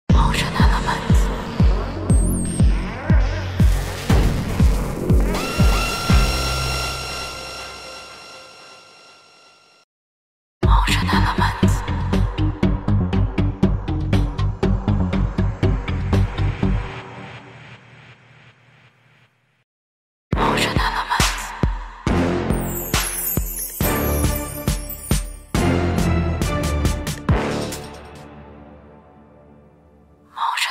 DJ VJ logo opener After Effects template
No plugins required - in main file is also version with prerendered
Optical Flare plugin
3 different versions like in preview
Video help included